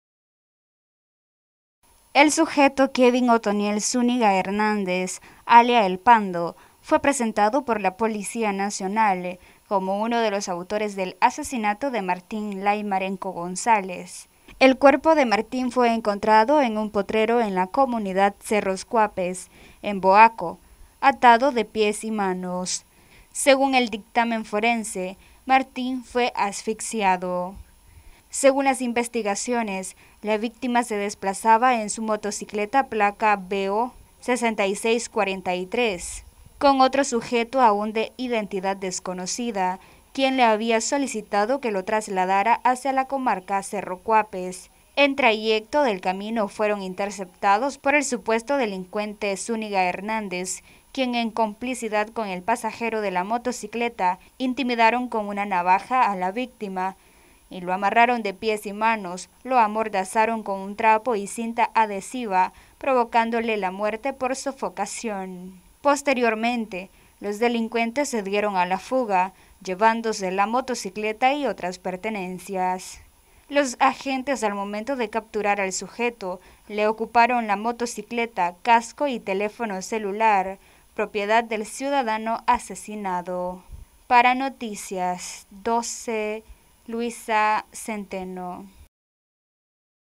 Policía Esclarece crimen de hombre que fue asesinado en Boaco el pasado 09 de septiembre
Noticias12Nicaragua Policía Esclarece crimen de hombre que fue asesinado en Boaco el pasado 09 de septiembre.